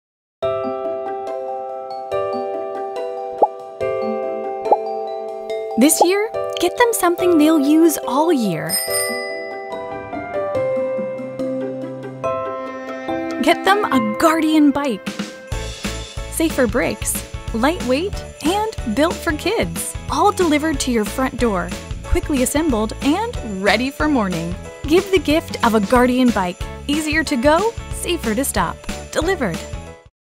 Gift That Lasts All Year | Guardian Bikes TV Commercial
A Guardian Bike is the toy that lasts all year.

Our mission is accident-free biking. All our bikes are equipped with our SureStop braking technology preventing head over handlebar accidents and giving your child a quicker more controlled stop.